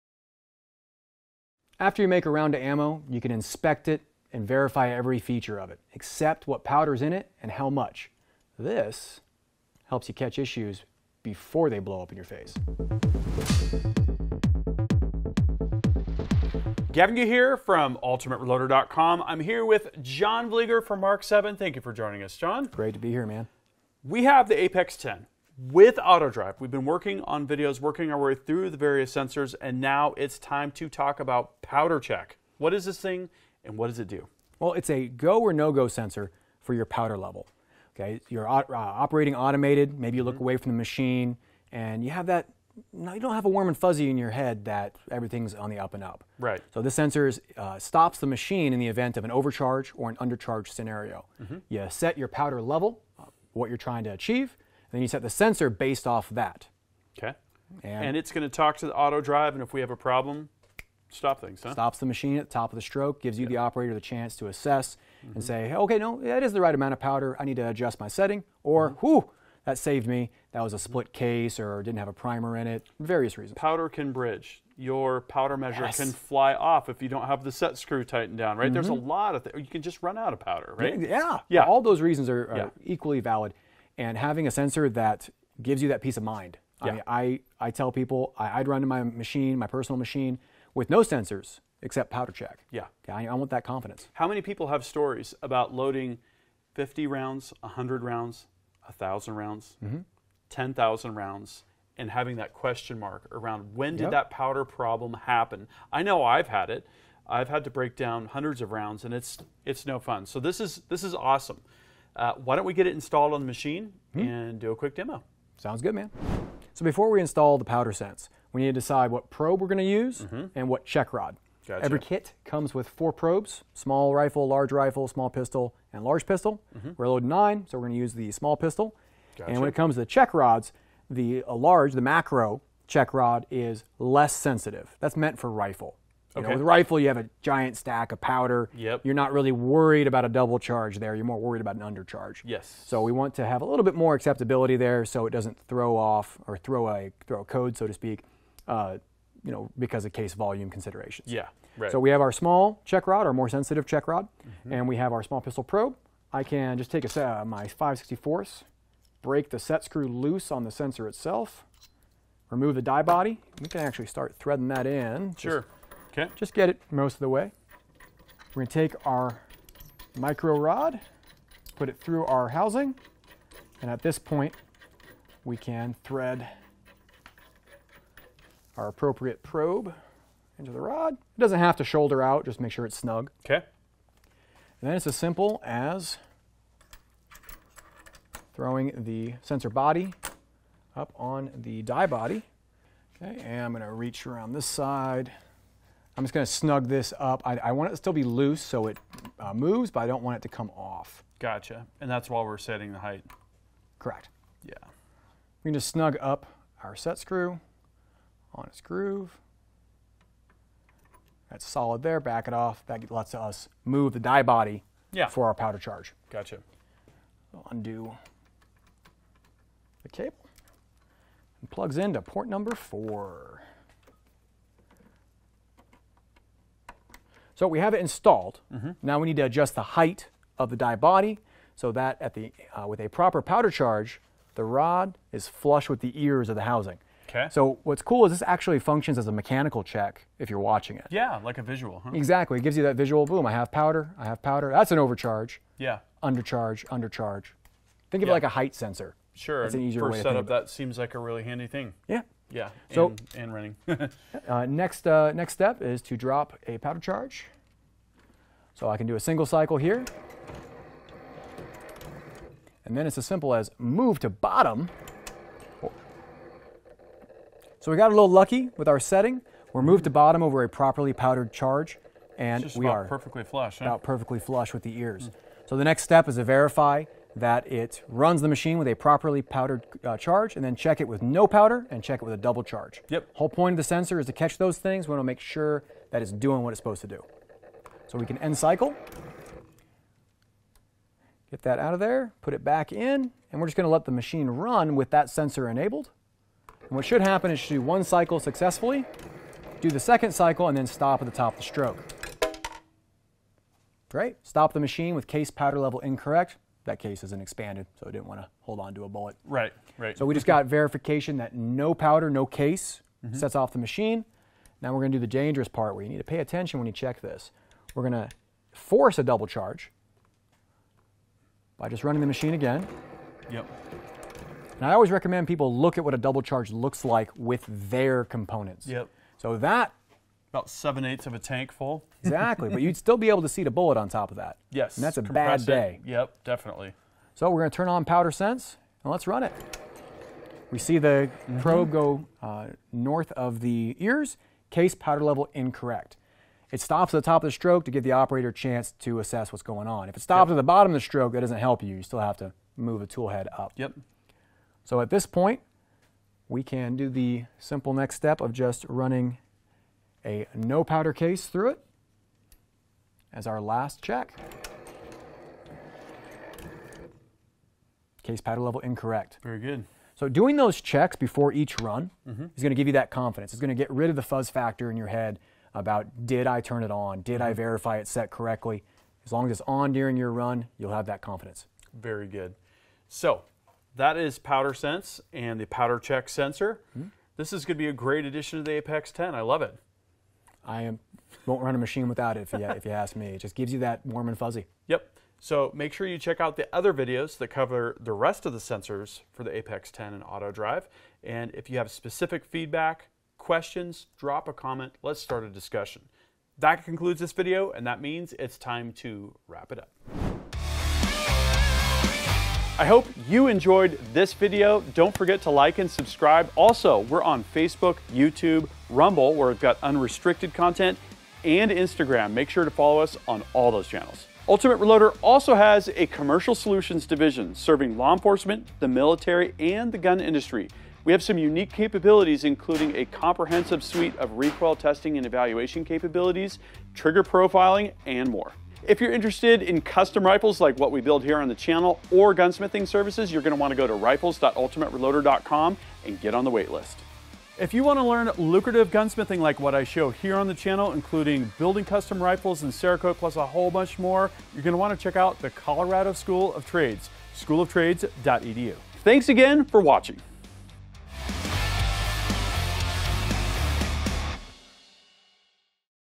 Mark 7 APEX 10: Electronic PowderCheck Sensor (Demo and Setup)
You can verify a number of features of a loaded round, but you can’t verify the amount or type of powder in it. John Vleiger from Mark 7 discusses how the electronic PowderCheck Sensor can help.

Ultimate Reloader LLC / gavintoobe Material Connection Notice:

The following Ultimate Reloader partners are featured in this video:
- Mark 7 Reloading